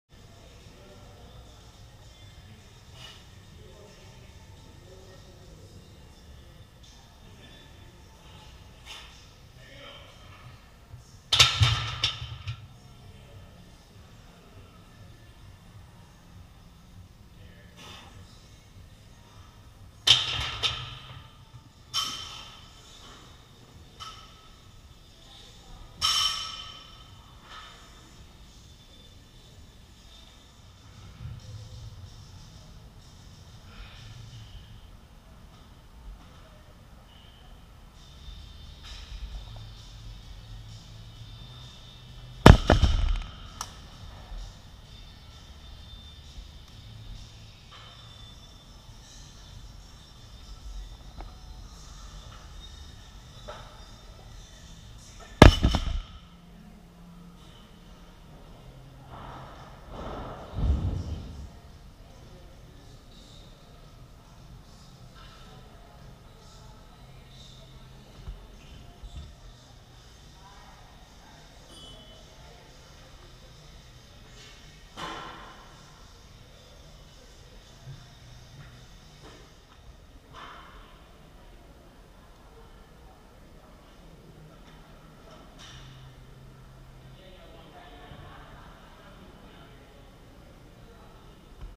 Off Season
Progress With new program. Working on speed. 3 months out IPL world